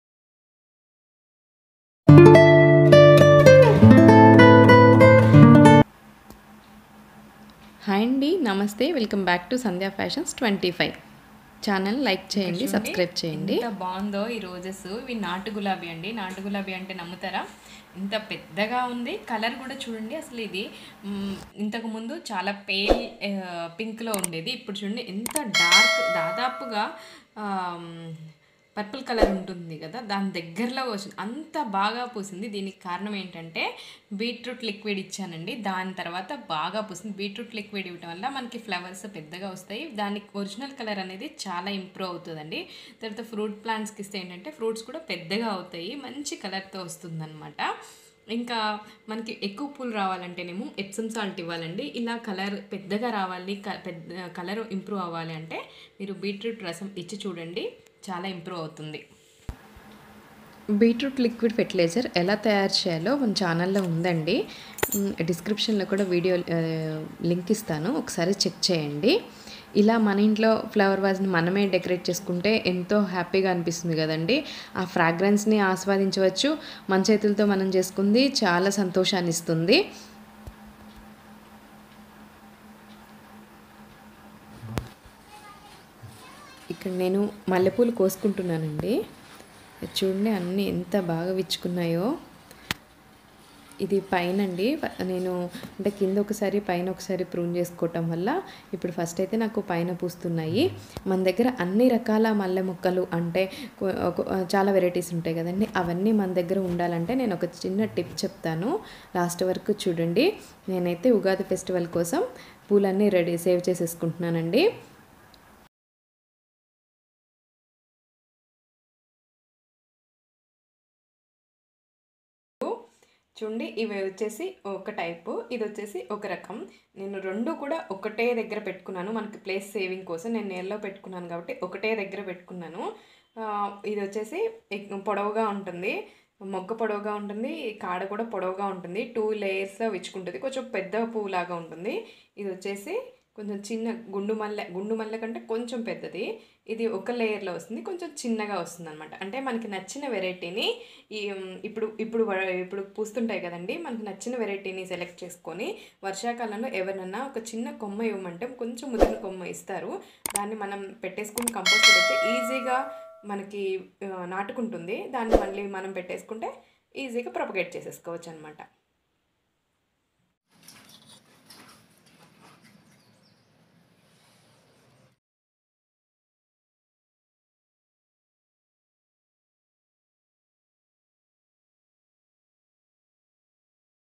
మంచి రంగు మరియు పెద్ద సైజు గులాబీలను, వివిధ రకాల మల్లె పూలను పొందడానికి ఈ చిట్కాను పాటించండి....
మంచి రంగు మరియు పెద్ద సైజు గులాబీలను పొందడానికి ఈ చిట్కాను పాటించండి | మీ తోటలో వివిధ రకాల మల్లె పూలను ఎలా పొందాలి.